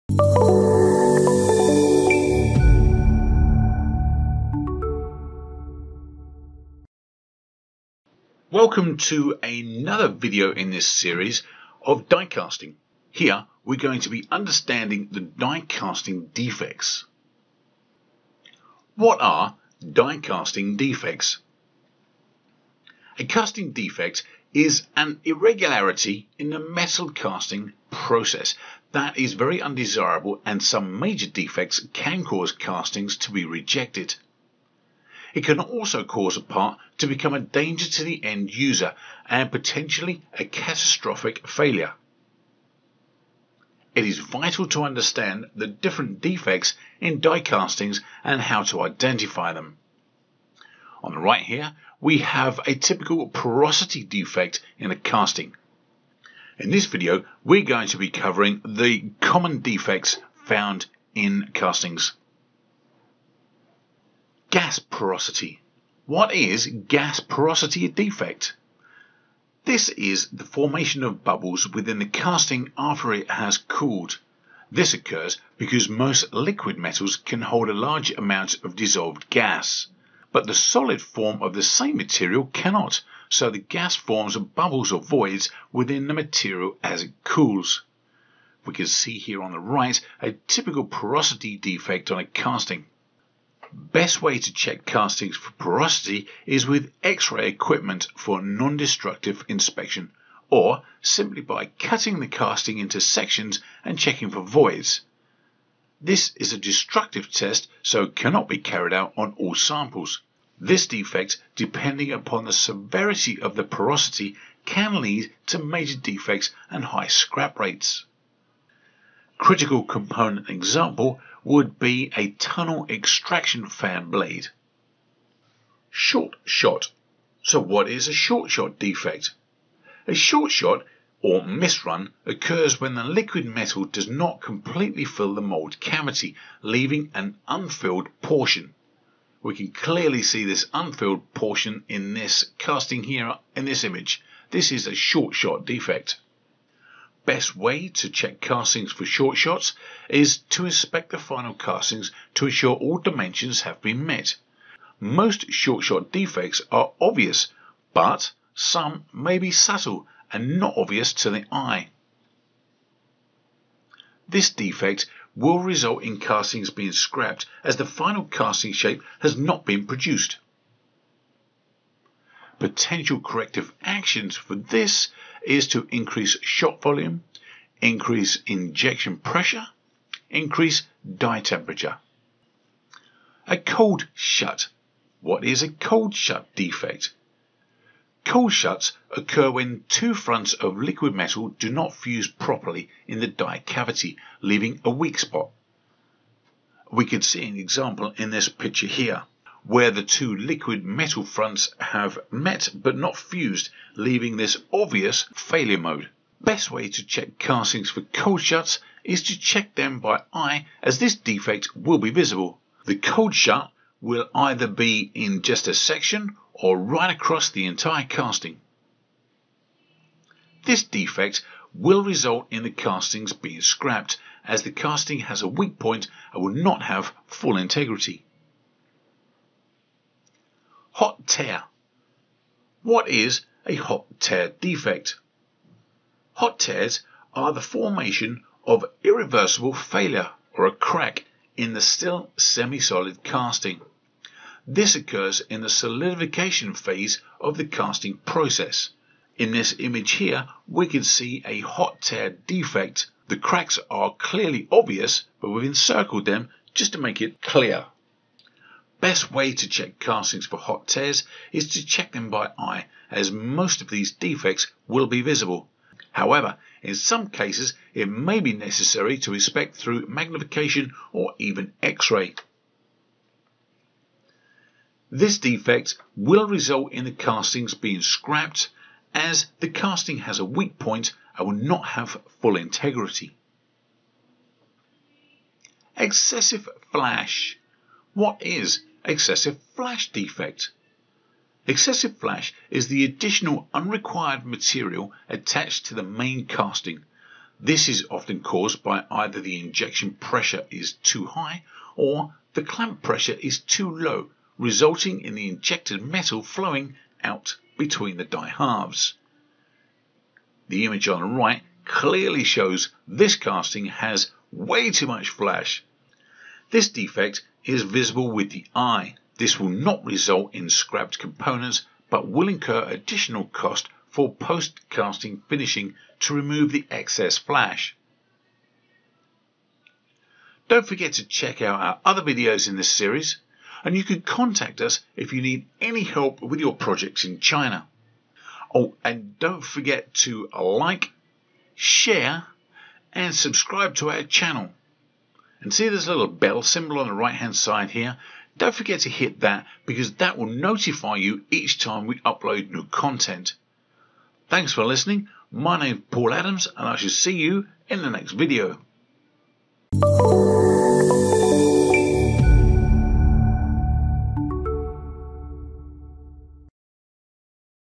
Understanding Die Casting Defects
Let's explore what the different kinds of die casting defects are, and how they're caused.

Not only do defects often cause die cast parts to be rejected, but they can also lead to dangerous products if not caught in time.

Let's look at what the following defects are, why they happen, and how to detect them:

- Gas porosity defects
- Short shots
- Cold shut defects
- Hot tears
- Excessive flash

Armed with this knowledge, you'll be better placed to assure that your die cast components and products are at the correct quality level that you require when checking them.

You may also like to learn more about the materials & processes that we're experienced in working with on this